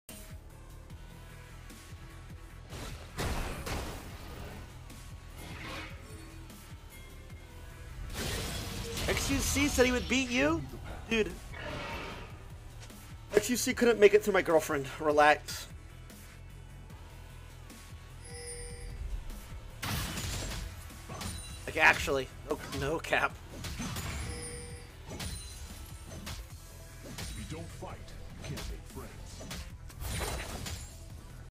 Tyler1 responds to xQc saying he would beat him
-If you Appreciate what I do and would like to Support me